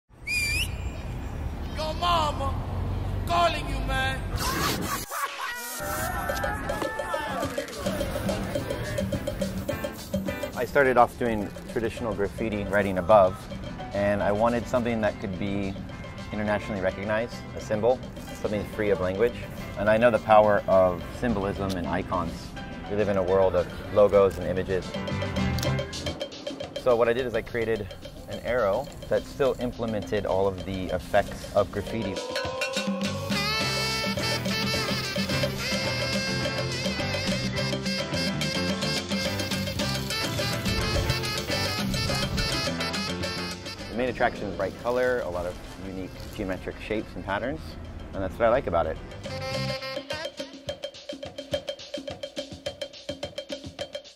ABOVE
Short Artist profile for Tavar and his collaboration for Zara Fashion and Wynwood Walls, Miami.